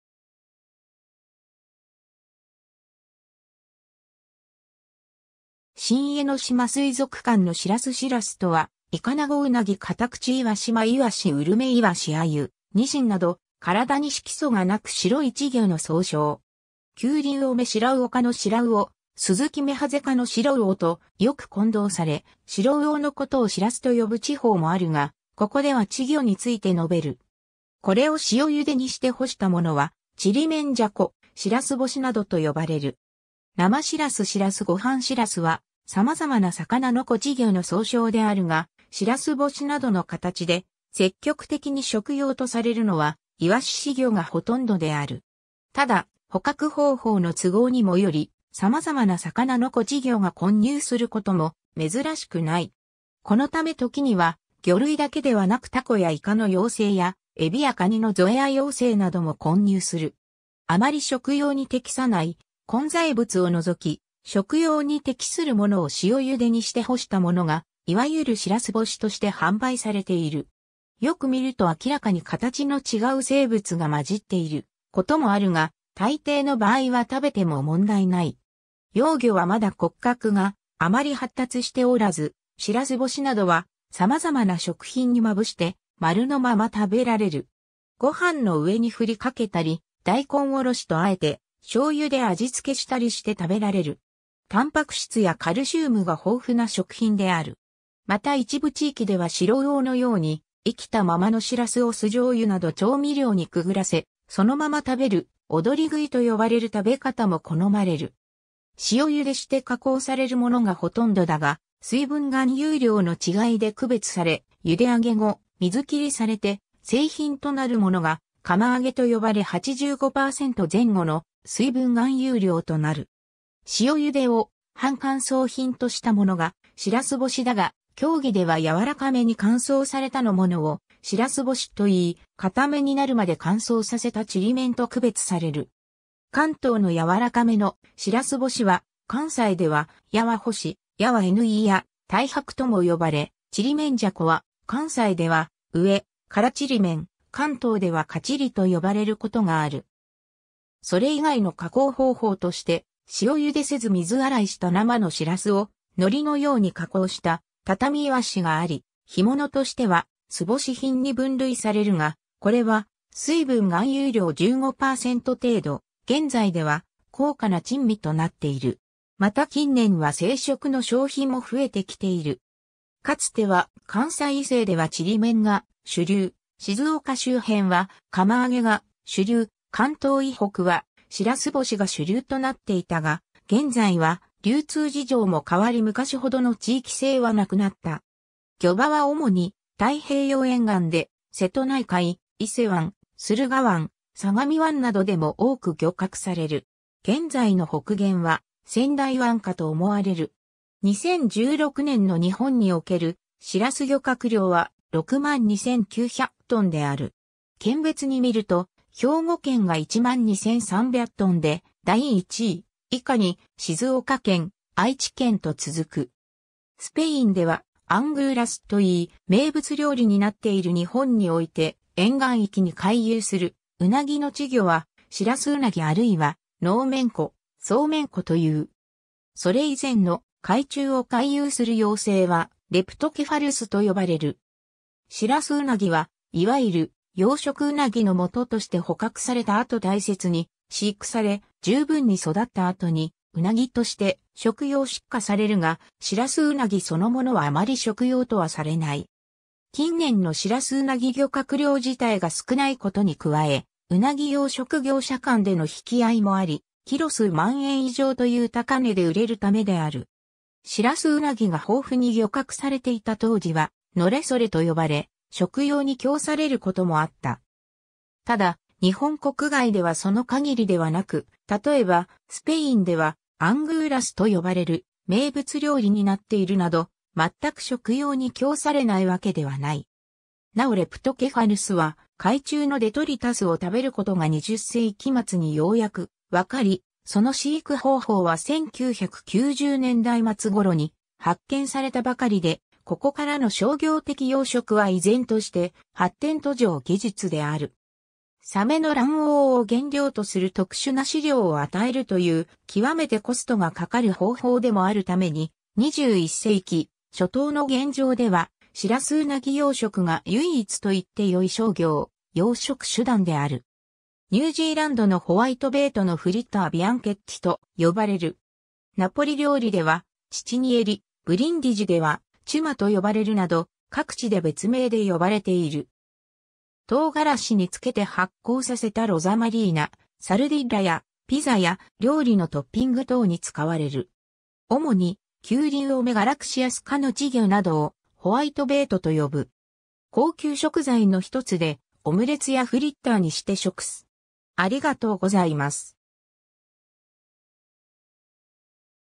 シラス (魚)
新江ノ島水族館のシラス シラス（白子、英: Whitebait）とは、イカナゴ・ウナギ・カタクチイワシ・マイワシ・ウルメイワシ・アユ・ニシンなど、体に色素がなく白い稚魚の総称。
キュウリウオ目シラウオ科のシラウオ（白魚）、スズキ目ハゼ科のシロウオ（素魚）とよく混同され、シロウオのことをシラスと呼ぶ地方もあるが、ここでは稚魚について述べる。
これを塩ゆでにして干したものは、ちりめんじゃこ、白子干し（白子乾し）などと呼ばれる。
 生シラス しらすごはん シラスは、様々な魚の仔稚魚の総称であるが、白子干しなどの形で積極的に食用とされるのは、イワシ仔魚（主にカタクチイワシ）がほとんどである。
ただ、捕獲方法の都合（シラス網と呼ばれる網を打って捕獲する）にもより、様々な魚の仔稚魚が混入することも珍しくない。
このため時には魚類だけではなくタコやイカの幼生やエビやカニのゾエア幼生（動物性プランクトンの一種）なども混入する。
 あまり食用に適さない混在物を除き、食用に適するものを塩茹でにして干したものがいわゆる白子干しとして販売されている。
よく見ると明らかに形の違う生物が混じっていることもあるが、大抵の場合は食べても問題ない。
 幼魚はまだ骨格があまり発達しておらず、白子干しなどは様々な食品にまぶして丸のまま食べられる。
ごはんの上にふり掛けたり（しらす丼）、大根おろしと和えて醤油で味付けしたりして食べられる。
蛋白質やカルシウムが豊富な食品である。
また一部地域ではシロウオのように、生きたままのシラスを酢醤油など調味料にくぐらせ、そのまま食べる「踊り食い」と呼ばれる食べ方も好まれる。
 塩茹でして加工されるものがほとんどだが、水分含有量の違いで区別され、茹で上げ後、水きりされて製品となるものが釜揚げと呼ばれ85%前後の水分含有量となる。
 塩茹でを半乾燥品としたものが（広義上の）「しらす干し」だが、狭義ではやわらかめ（水分率50–85%）に乾燥されたのものを「しらす干し」（関東干し）といい、固め（水分率30–50%弱程度）になるまで乾燥させたちりめん（関西干し）と区別される。
 関東のやわらかめの「しらす干し」は、関西では「やわ干し、やわ乾」や「 太白」とも呼ばれ、ちりめんじゃこは、関西では「上乾（じょうぼし）ちりめん」、関東ではかちりと呼ばれることがある。
 それ以外の加工方法として、塩ゆでせず水洗いした生のシラスを海苔のように加工した畳いわし（たたみいわし）（枠に入れ四角い薄板状に成形して乾燥させたもの）があり、干物としては「素干し品」に分類されるが、 これは水分含有量15%程度、現在では高価な珍味となっている。
また近年は生食の消費も増えてきている。
 かつては関西以西ではちりめんが主流、静岡周辺は釜揚げが主流、関東以北はシラス干が主流となっていたが、現在は流通事情も変わり昔ほどの地域性はなくなった。
 漁場は主に太平洋沿岸で、瀬戸内海、伊勢湾、駿河湾、相模湾などでも多く漁獲される。
現在の北限は仙台湾かと思われる。
 2016年の日本におけるシラス漁獲量は、62900トンである。
県別に見ると、兵庫県が12300トン (19.6 %) で第1位、以下に静岡県（8900トン、14.1 %）、愛知県（8400トン、13.4 %）と続く。
 スペインでは「アングーラス(angulas)」といい、名物料理になっている 日本において沿岸域に回遊するウナギの稚魚は「シラスウナギ」あるいは「ノウメンコ」、「ソウメンコ」という。
それ以前の、海中を回遊する幼生はレプトケファルス（葉形仔魚）と呼ばれる。
シラスウナギはいわゆる「養殖ウナギ」の元として捕獲された後大切に飼育され、十分に育った後にウナギとして食用出荷されるが、シラスウナギそのものはあまり食用とはされない。
近年のシラスウナギ漁獲量自体が少ないことに加え、ウナギ養殖業者間での引き合いもあり、キロ数万円以上（2000年頃）という高値で売れるためである。
シラスウナギが豊富に漁獲されていた当時は「のれそれ」と呼ばれ食用に供されることもあった。
 ただ日本国外ではその限りではなく、例えばスペインでは「アングーラス(angulas)」と呼ばれる名物料理になっているなど、全く食用に供されない訳ではない。
 なおレプトケファルスは海中のデトリタスを食べることが20世紀末にようやくわかり、その飼育方法は1990年代末頃に発見されたばかりで、ここからの商業的養殖は依然として発展途上技術である。...